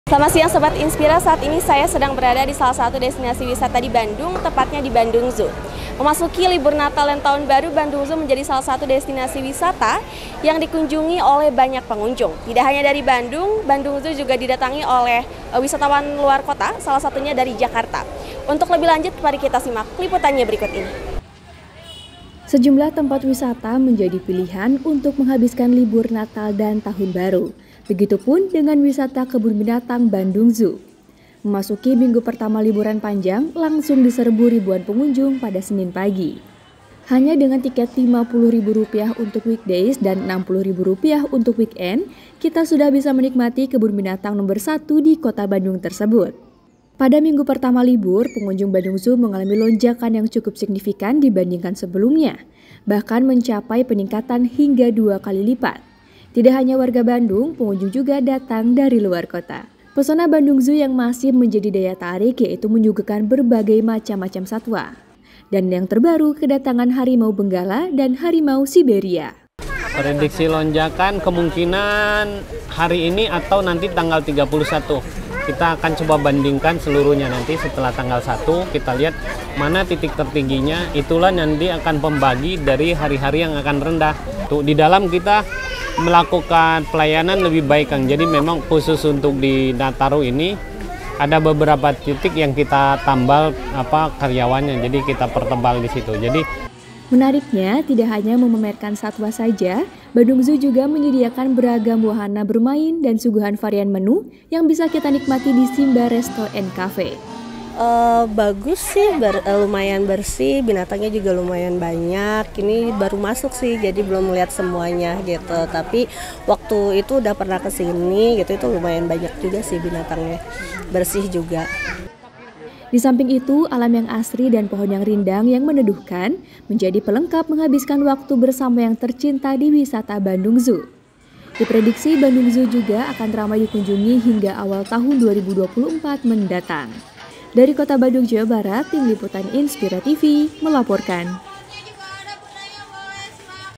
RIBUAN WARGA KUNJUNGI BANDUNG ZOO KOTA BANDUNG
INSPIRA TV-Memasuki Libur Natal Dan Tahun Baru, Kebun Binatang Bandung Atau Bandung Zoo Menjadi Salah Satu Destinasi Wisata Yang Diburu Pengunjung, Buka Mulai Pukul 10.00 Wib, Bandung Zoo Menyuguhkan Beragam Jenis Satwa.